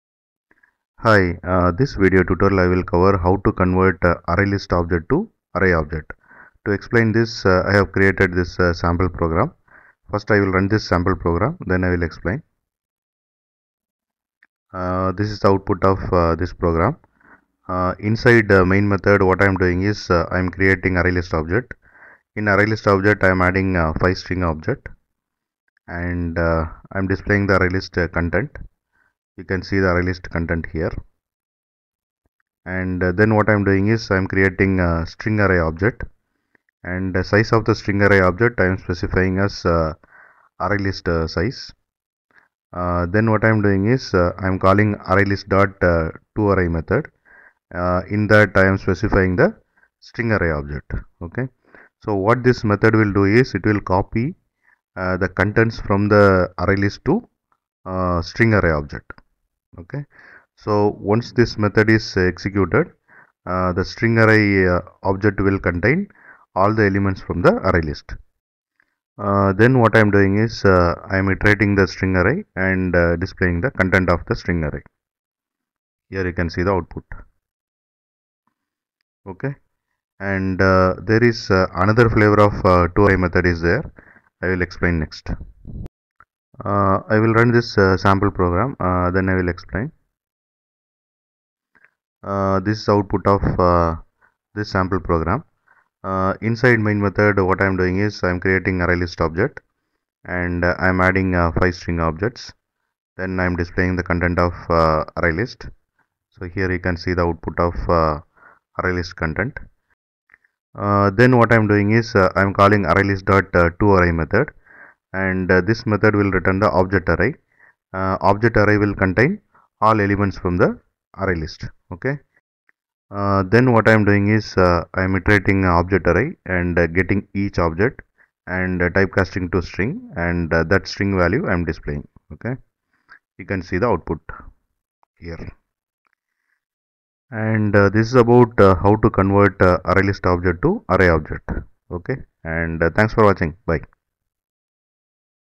How to Convert an ArrayList object to an Array object? | Java Collection Framework
Welcome to this Java tutorial where we'll explore how to convert an ArrayList object to an Array object using the Java Collection Framework!

In Java programming, you may often need to convert an ArrayList to a regular array for various purposes, such as compatibility with legacy code or specific API requirements. Fortunately, the `toArray()` method provided by the ArrayList class allows us to perform this conversion efficiently.

In this video, we will cover the following topics:
- **Understanding ArrayList to Array Conversion**: Learn about the `toArray()` method and how it can be used to convert an ArrayList to an array.
- **Syntax and Usage**: Explore the syntax of the `toArray()` method and understand how to use it to obtain an array representation of an ArrayList.
- **Conversion Example**: Walk through practical examples demonstrating how to convert an ArrayList of objects to an array of specified type.
- **Handling Generic and Object Arrays**: Discuss approaches for converting ArrayLists containing generic or specific types of objects to corresponding arrays.

Throughout the tutorial, we'll provide clear code examples and explanations to help you grasp the concept of ArrayList to Array conversion in Java. By the end of this video, you'll have the skills to effectively convert ArrayList objects to Array objects in your Java projects.

Let's dive into ArrayList to Array conversion and enhance our Java skills together! Happy coding!